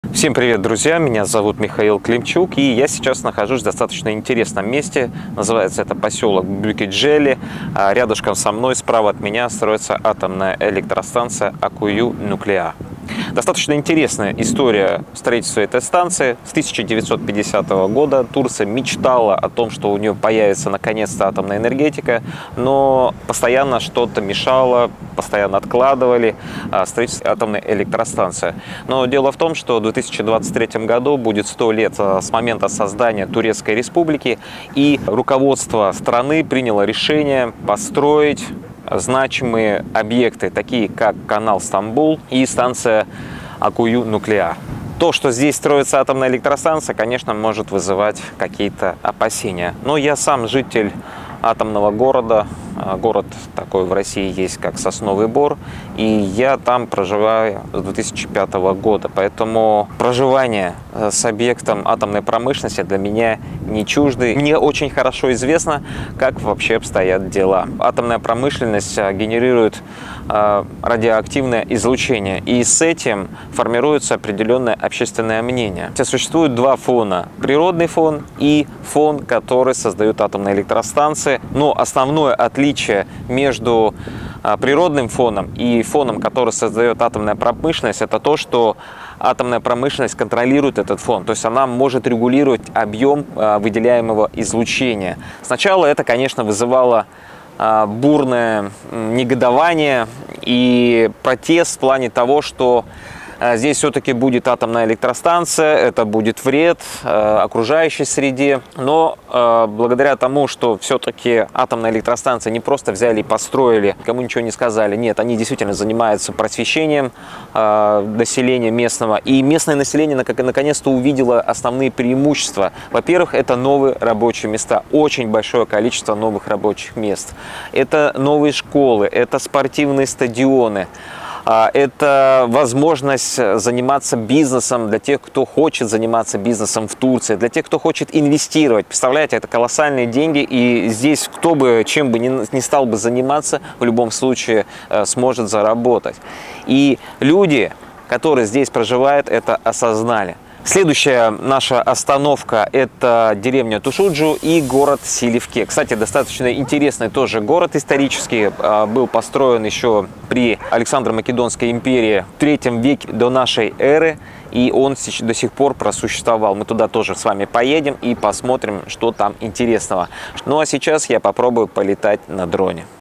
Поселок Буюкеджели. Атомная станция Аккую Нуклеар. Турция
В середине 1950-х годов президент США Дуайт Эйзенхауэр заключил с Турцией соглашение о сотрудничестве в рамках американской программы «Атом для мира».
Но дальше разработки ТЭО дело не пошло, и долгое время мечты Турции о собственной ядерной энергетике оставались мечтами. Причины были разными: то смена политических режимов в стране, то отказ правительства предоставить гарантии финансирования, то чернобыльский синдром, то деятельность «зеленых».

С 1970-х Турция занималась поиском места для станции. Были исследованы побережья Черного моря, после чего выбрали площадку в провинции Синоп. Но наиболее безопасным с точки зрения сейсмоактивности был признан участок в Мерсине».

В 1976 году на площадку в Аккую была выдана лицензия. И снова все застопорилось. Несколько раз объявляли тендер, но не доводили до конца. Так продолжалось вплоть до 2010 года, пока не было подписано межправительственное соглашение с Россией.

Был подготовлен отчет по оценке воздействия на окружающую среду, сельское хозяйство, рыболовство, туризм, промышленность и многое другое. Это огромный документ – около 5,5 тыс. страниц. На его разработку и согласование ушло два с половиной года.

Чтобы просто подготовить площадку, пришлось сравнять с землей гору. «За четыре года было перемещено свыше 4,5 млн м3 грунта. Площадка находится под неусыпным контролем. Специалисты из России согласовывают любое действие с Министерством энергетики и природных ресурсов, Министерством окружающей среды и градостроительства, Министерством лесного хозяйства и другими министерствами и ведомствами.

Постоянно ведется мониторинг сейсмоактивности, уровня естественной радиации и температуры окружающей среды. Полученные данные будут накапливаться и анализироваться на протяжении всего жизненного цикла АЭС. Проект станции полностью соответствует постфукусимским требованиям. «В этом регионе сейсмичность порядка 7 баллов, но блоки смогут выдержать до 9 баллов. По хребтам гор проходят пожарные просеки, чтобы в случае чего перекрыть путь огню. Будет построена мощная дамба для защиты от цунами.